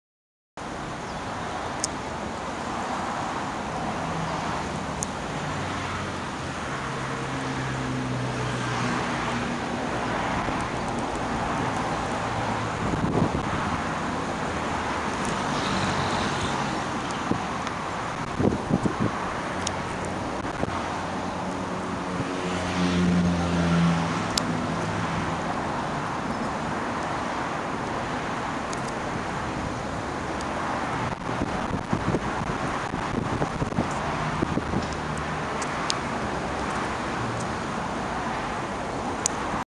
UNDER RIVER ATTACK
The oldest and most famous bridge of Rome Milvio Bridge under the attack of rain